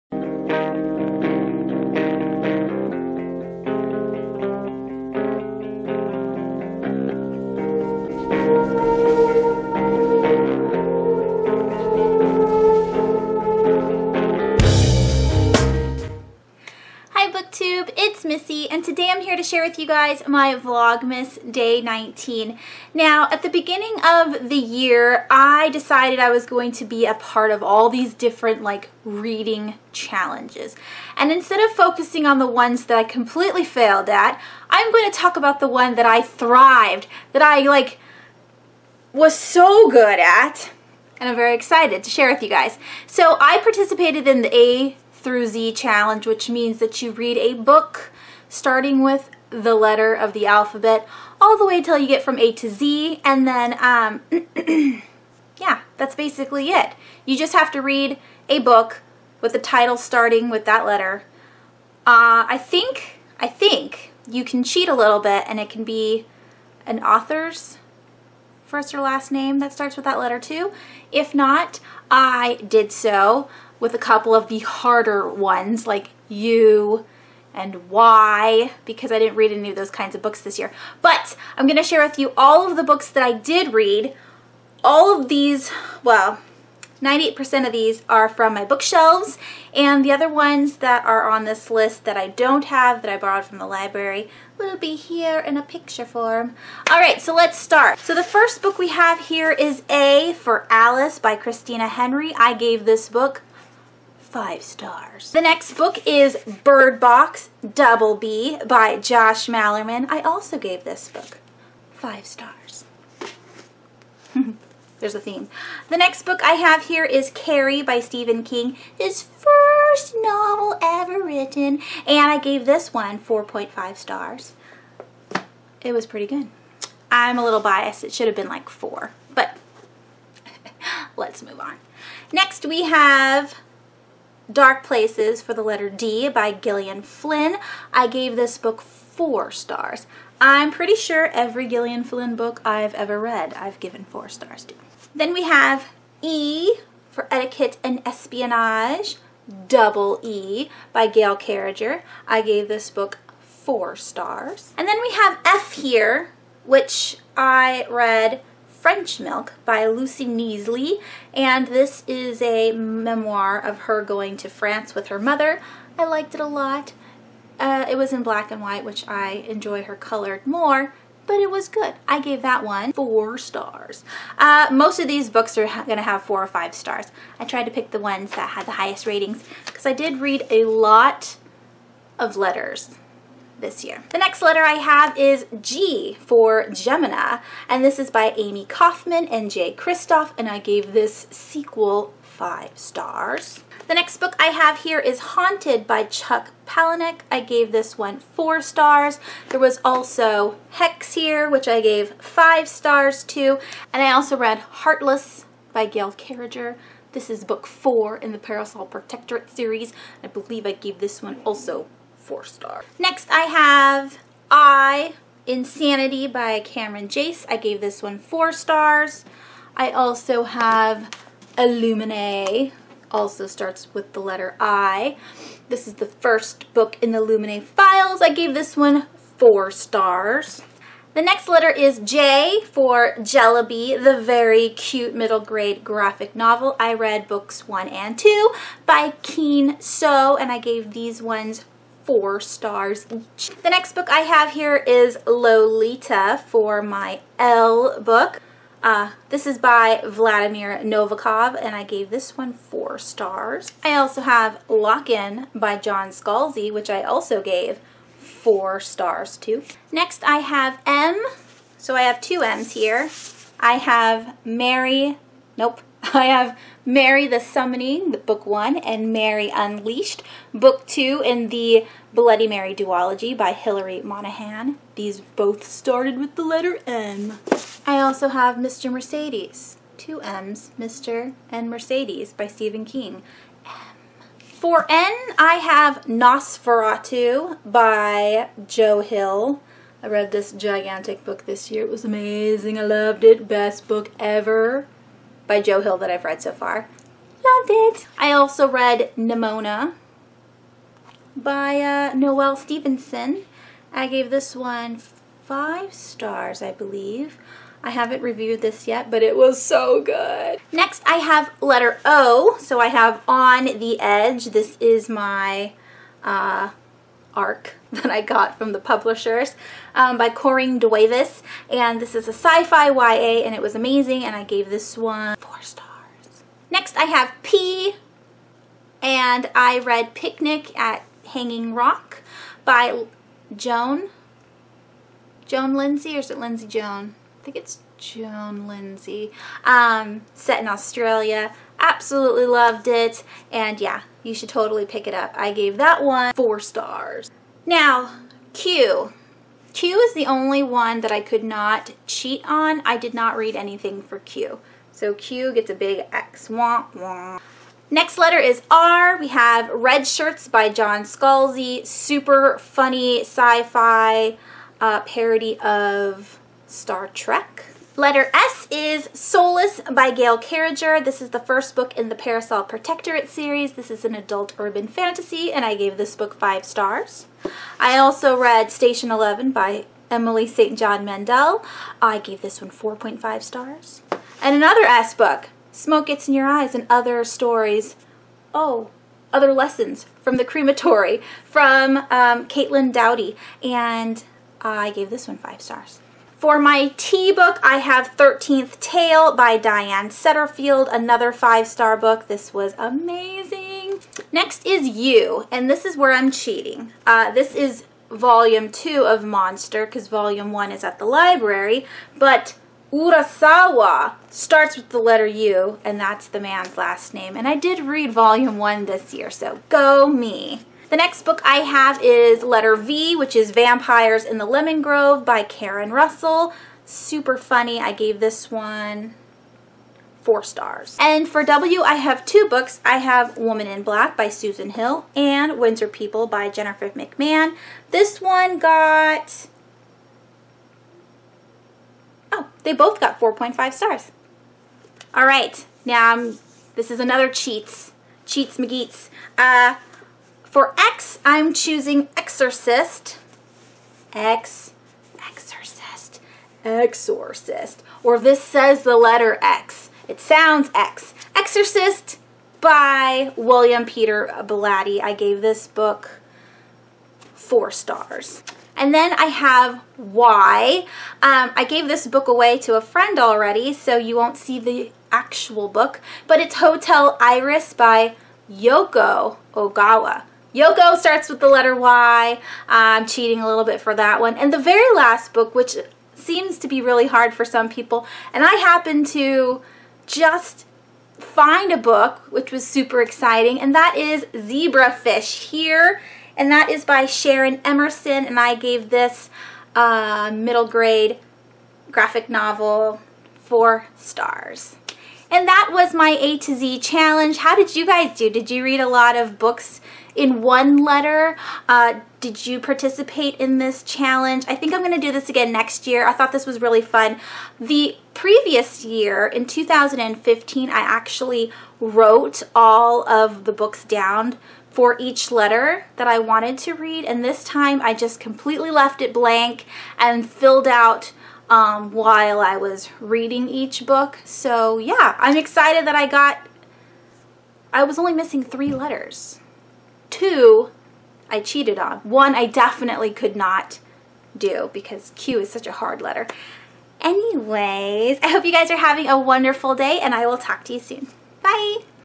A to Z Challenge Wrap Up || Vlogmas Day nineteen
Hi bookland :) What kind of reading challenge did you participate in this year? I was surprised at how many letters I got through :D

If you want to send me a book to swap
or whatever ;)